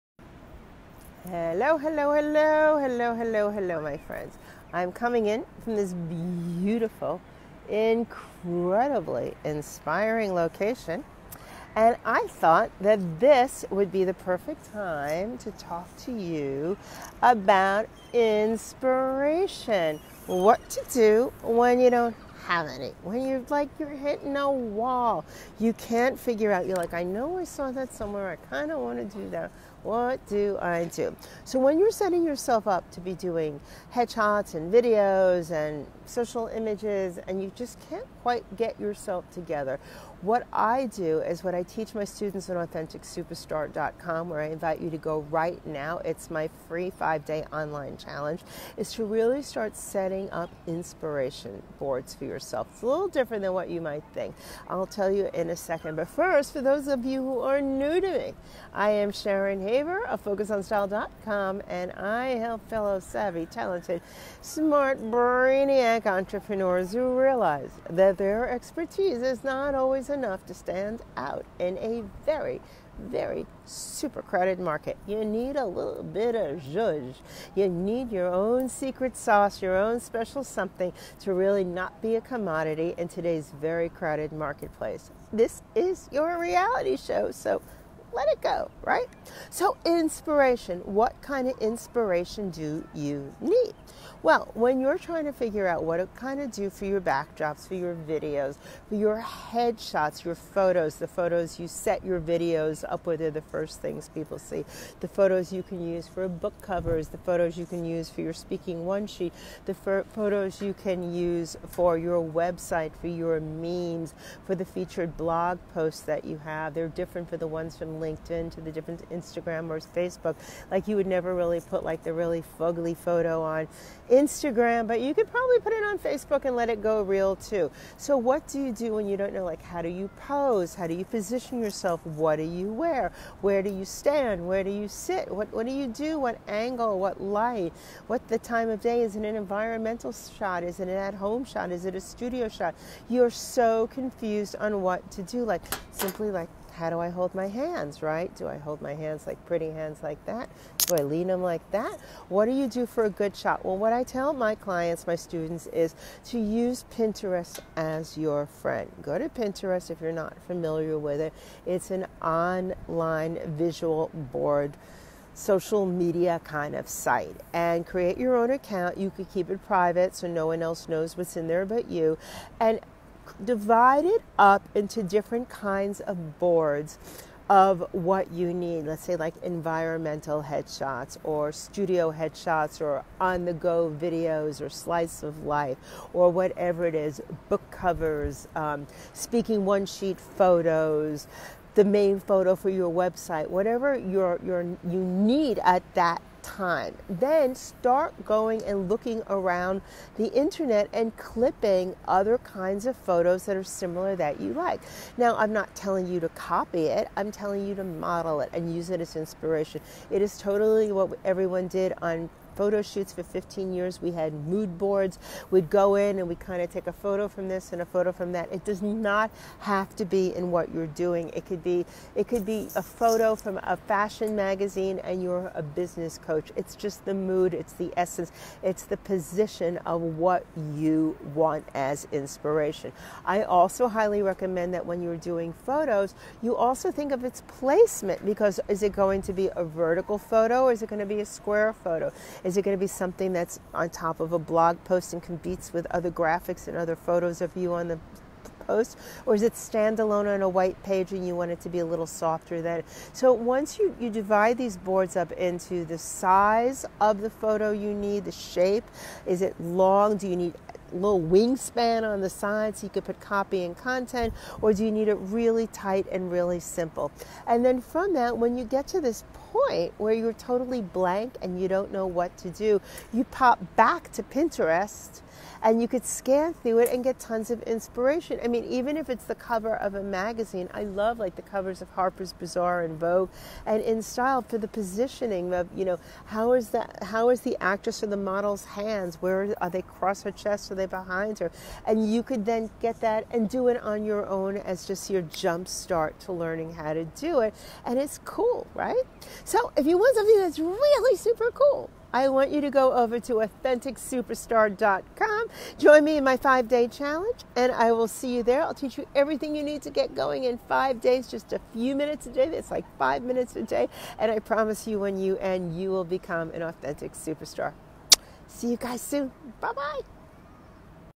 USE THIS TO SPARK INSPIRATION | And How To Keep It Up [LIVE at La Jolla]
Inspiration can be tricky.
But I know how you can get it!
This method I use WORKS and (bonus!) it's fun!
Join my challenge at AuthenticSuperstar.com to put this into action.